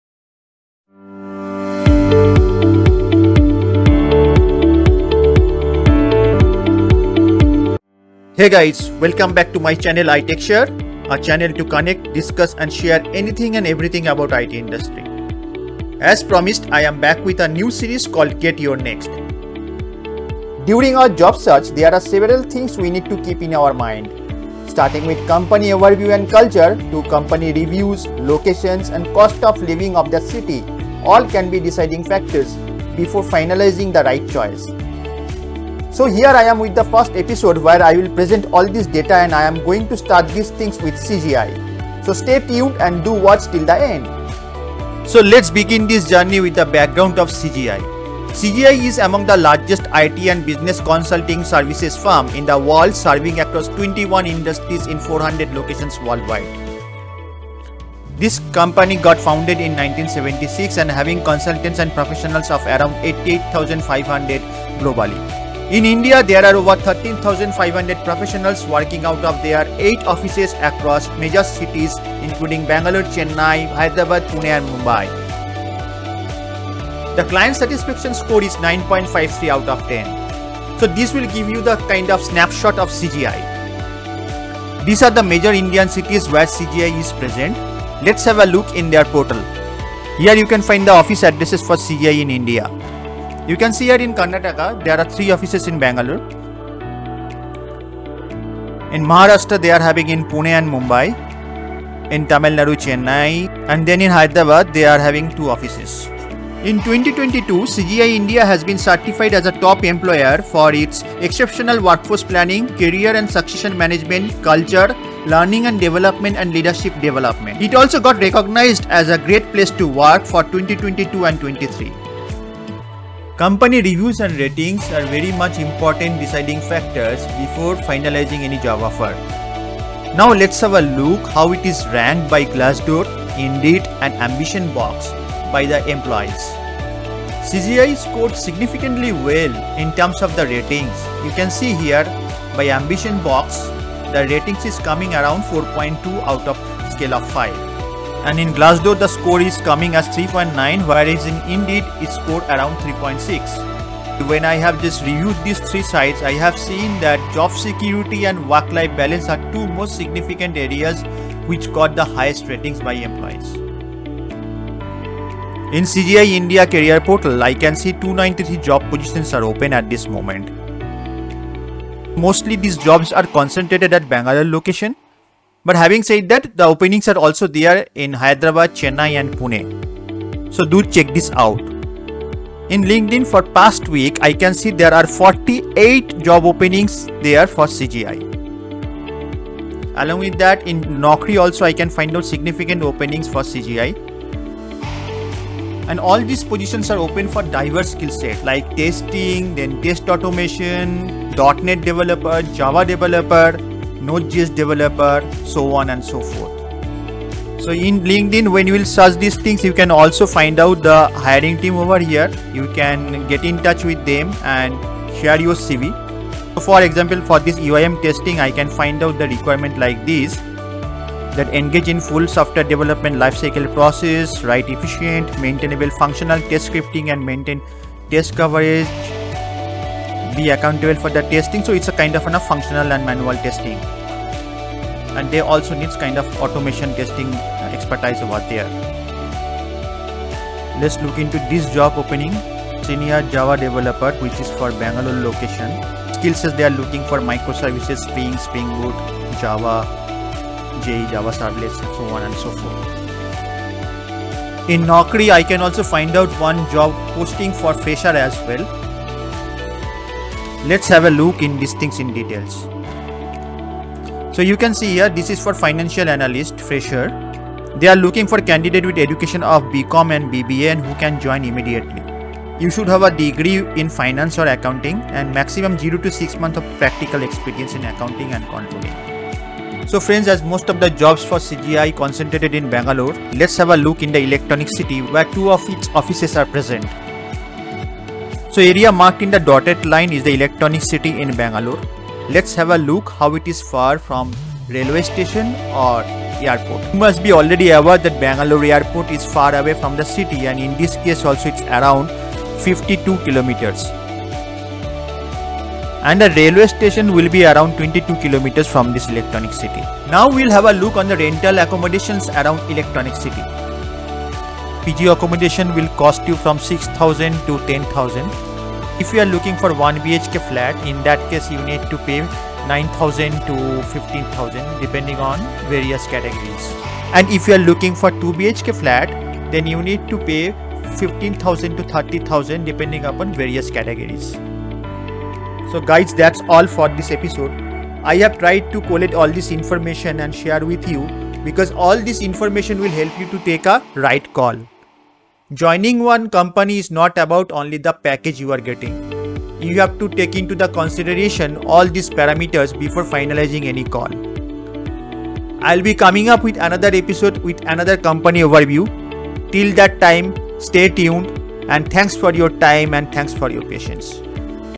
Get Your Next - EP1 :CGI is hiring for multiple positions | Job Vacancies, Location & other details
Job is not all about the only pay package. Before finalizing any offer, we need to get to understand about company background, location , cost of living etc. This is the first episode of Get Your Next Series, where I have discussed about CGI and current job vacancies. I have also discussed additional parameters which will help you to make the right choice.